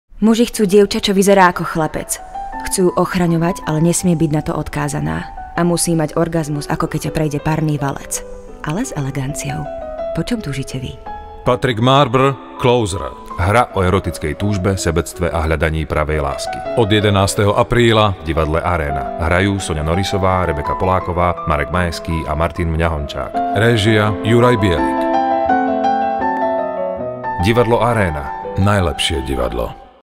CLOSER (Na dotyk) TV SPOT
Hra o erotickej túžbe, sebectve a hľadaní pravej lásky. réžia: Juraj Bielik, hrajú: Soňa Norisová, Rebeka Poláková, Marek Majeský, Martin Mňahončák. scéna a kostými: Marija Havran